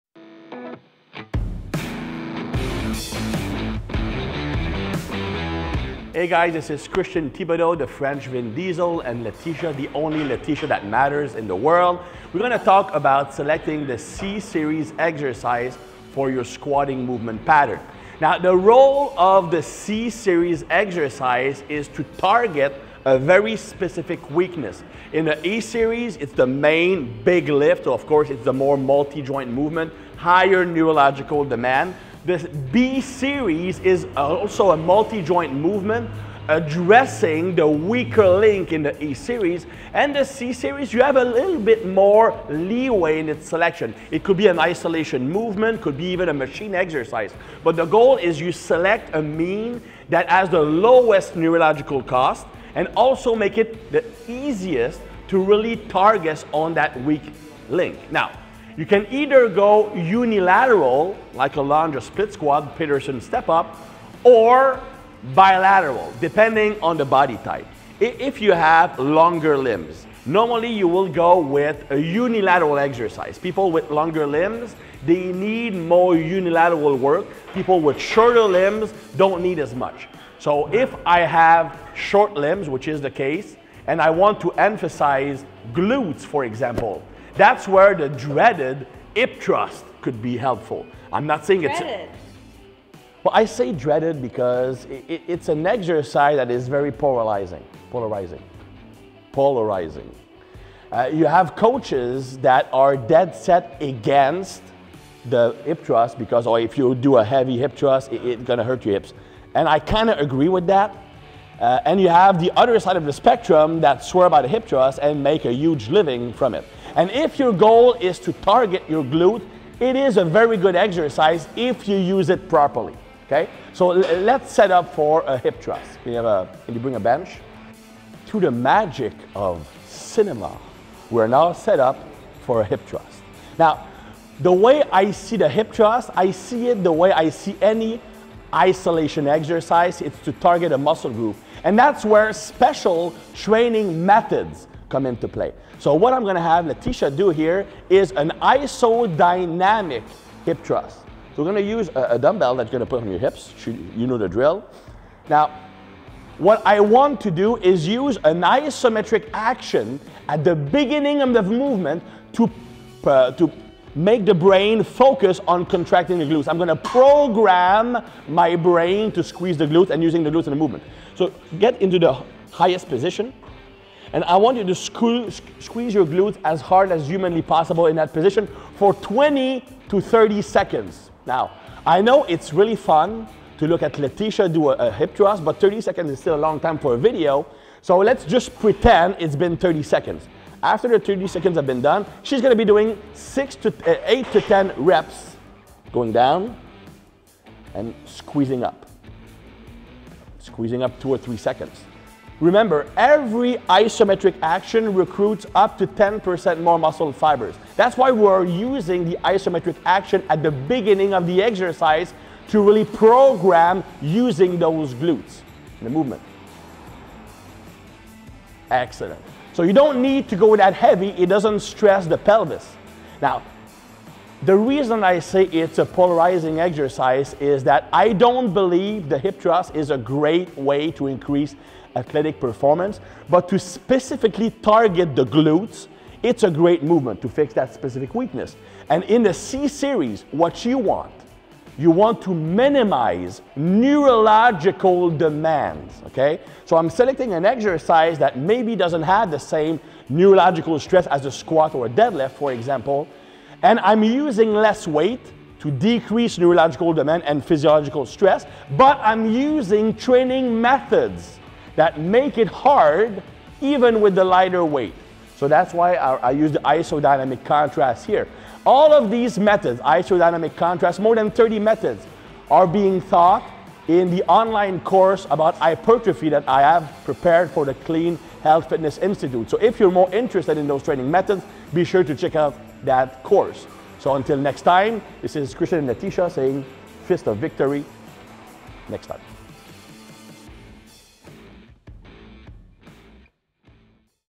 How to Build Bullet Proof Glutes with Christian Thibaudeau
Shot in 2019 at our company HQ with world-leading Strength Coach Christian Thibaudeau. In this video, he discusses how to improve your client's squat in as short a time as possible by working on glute development in your C series.